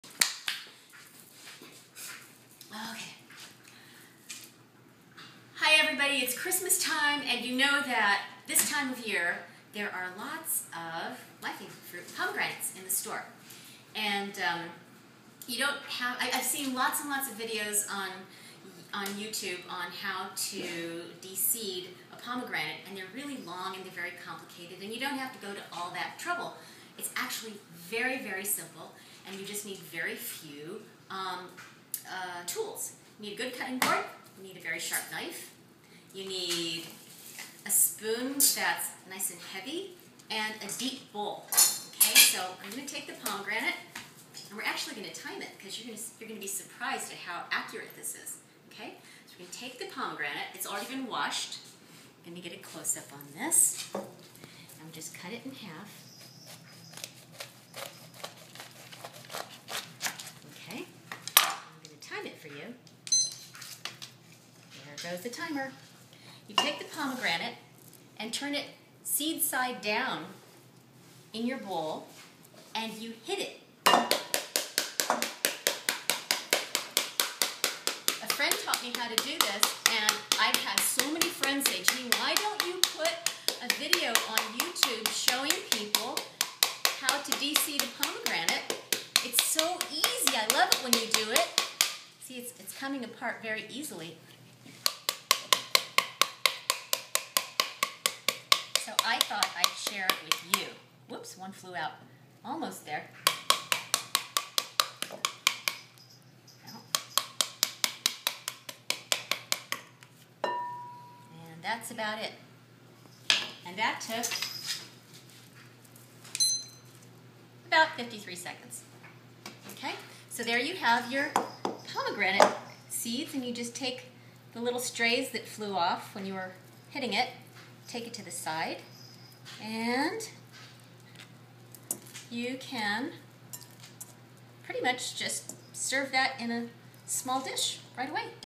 Easiest way to open a pomegranate!
Fastest, simplest, way to de-seed a pomegranate, with the help of a curious little kitten.
All my friends have urged me to do this video because all other pomegranate demonstrations are ridiculously difficult.
This is so simple, you'll have no reason not to eat pomegranates all winter long. ❤